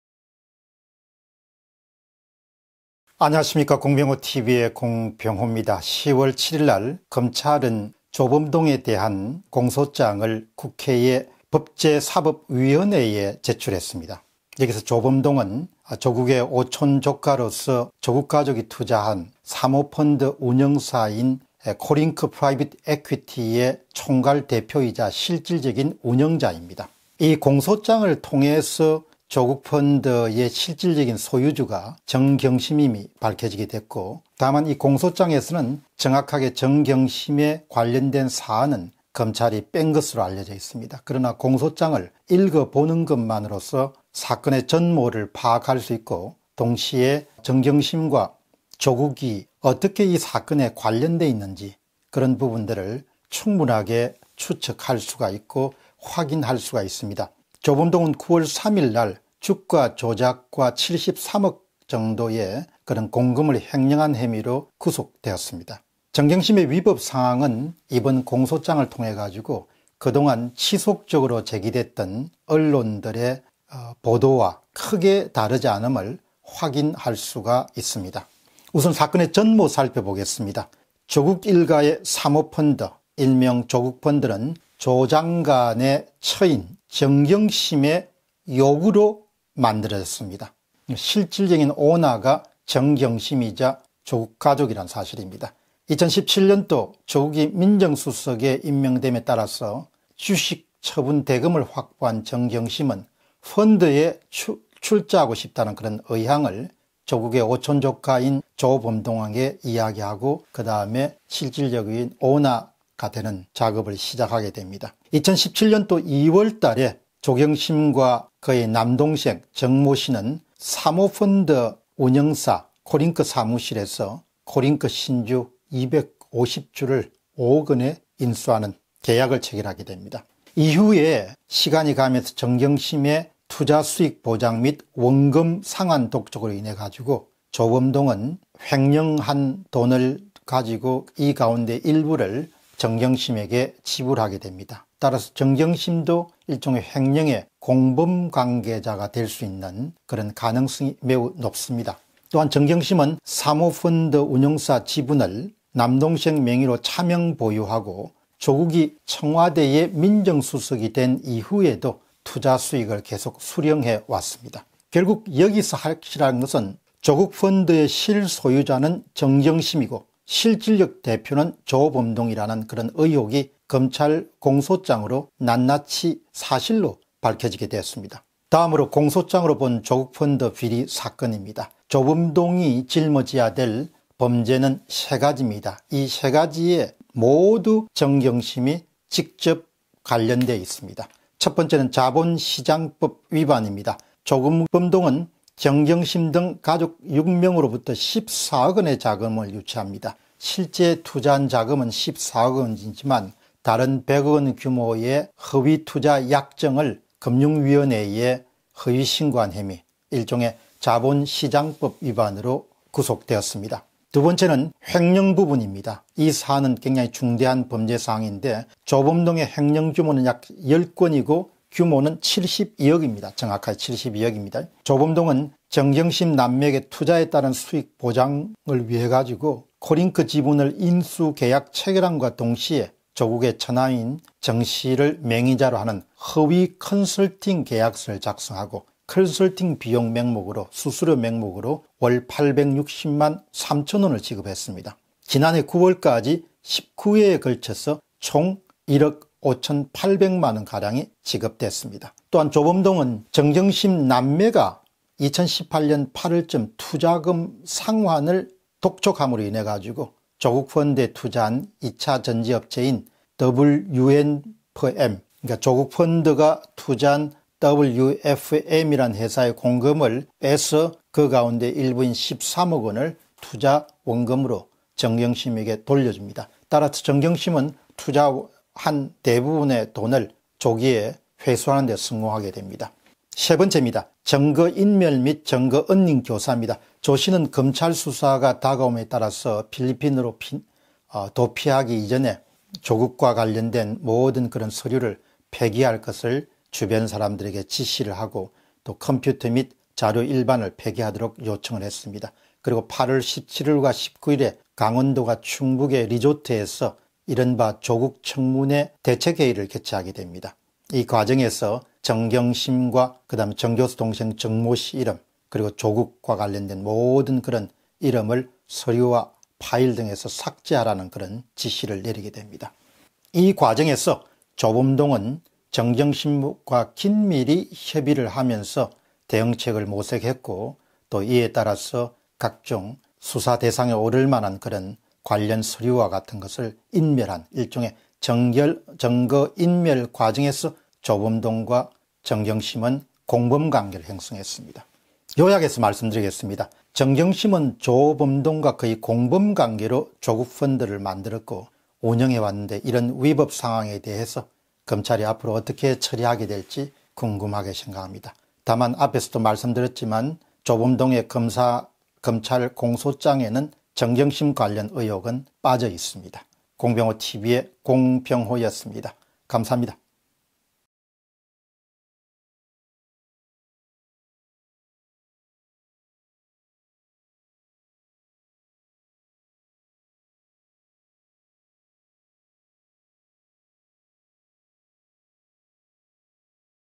정경심 사건 / 조범동 공소장 공개 [공병호TV]
정경심 사건 / 조범동 공소장 공개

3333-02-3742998 카카오은행 (예금주 : 공병호) / (텔레뱅킹 : 090 & 은행코드 : 90)

참고자료
1. 정경심, 남동생 명의로 코링크 차명투자, 안대규, 한국경제신문, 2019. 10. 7
2. 정경심, 코링크 차명투자 ... 민정수석 임명뒤에도 수익금 받아, 한국경제신문, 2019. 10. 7